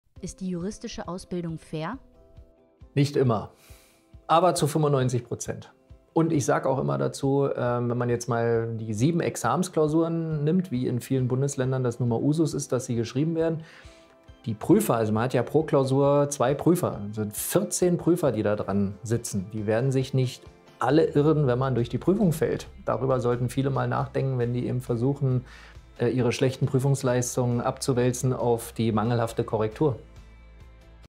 Ist Jura fair?
Wissenswertes und / oder Unterhaltsames in ungefähr einer Minute präsentieren? Diesem Unterfangen widmen sich die Akademie Kraatz und die Assessor Akademie in ihrem neuen Videoformat der "Jura-Shorts".

Jura ist sicherlich einer der schwersten Studiengänge überhaupt. Umso wichtiger ist es daher, dass die  Unis dazu in der Lage sind, den Studenten ein gutes und faires Studium zu ermöglichen. Ist dies der Fall? RA Mario Kraatz schildert seine Erfahrungen.